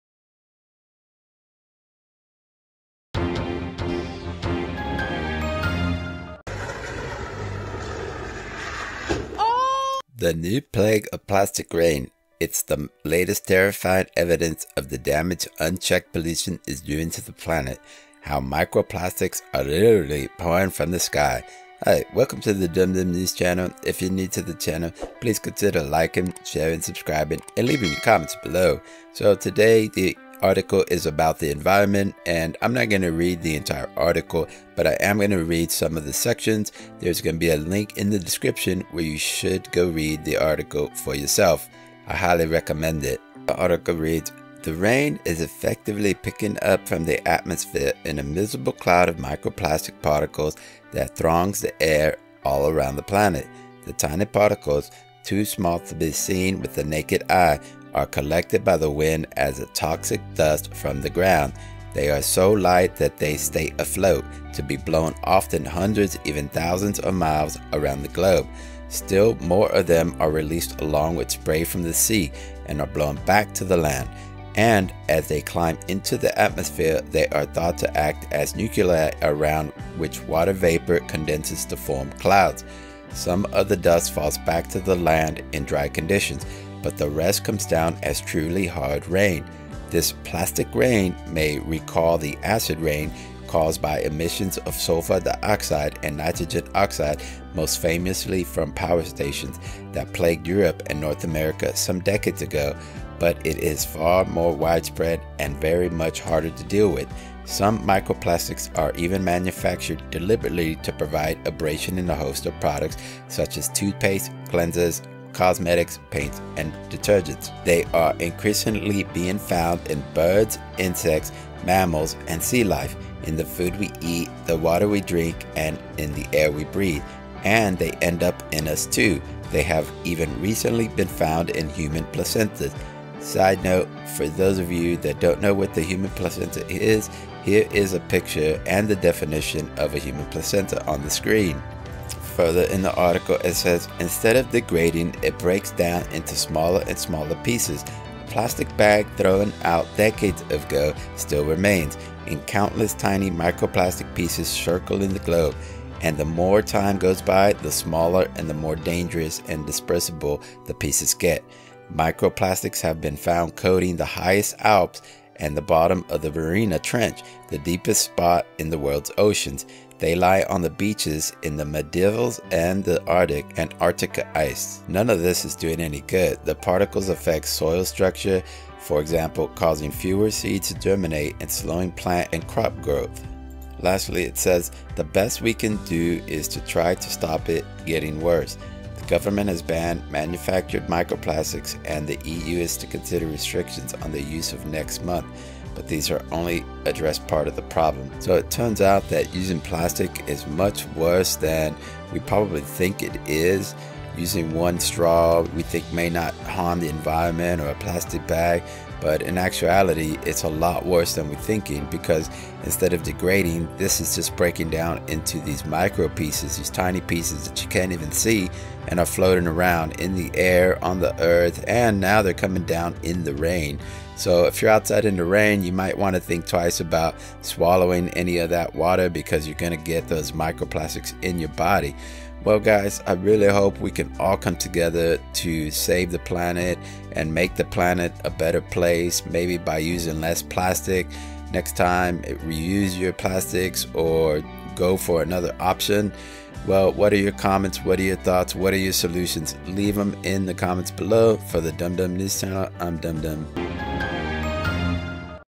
Plastic Rain, Microplastics Pouring Down Damage The Environment.
Plastic Rain: How Microplastics Are Literally Pouring From The Sky And Evidence Of The Damage By Pollution.

Dum Dum

✭ Captioning ✭
News broadcaster Dum Dum, “Hi, I'm your host Dum Dum and welcome to the Dum Dum News Channel. In today's news we'll be covering Plastic Rain: How Microplastics Are Literally Pouring From The Sky And Evidence Of The Damage By Pollution."